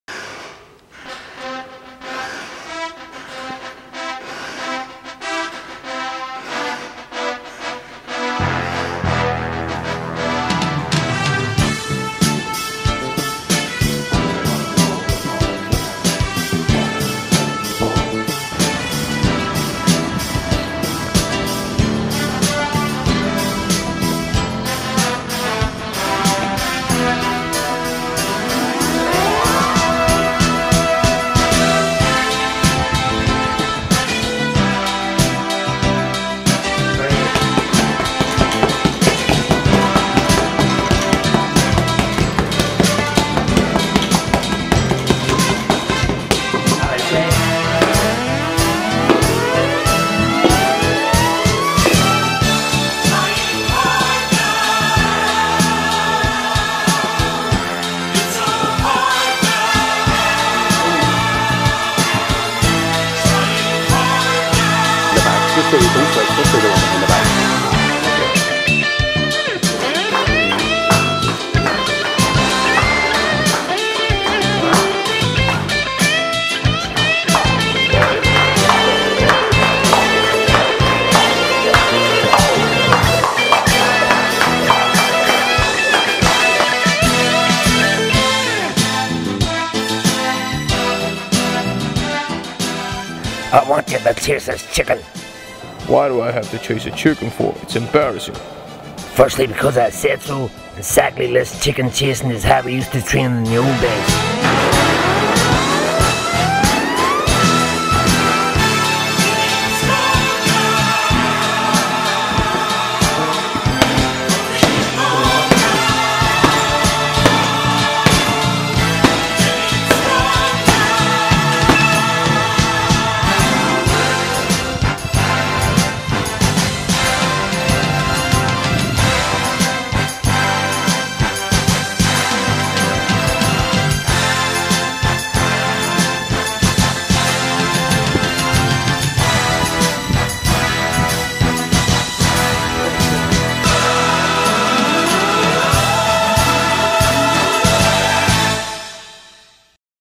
CROCKY II - BABHTA A DÓ
Sinn Féin ag tacú leis an Lá Dearg Dé Sathairn, 20ú Bealtaine, ar a 12 ag an Chultúrlann i mBéal Feirste.

Sinn Féin supporting an Lá Dearg demo in Belfast on Saturday 20th May, beginning at 12 at the Cultúrlann.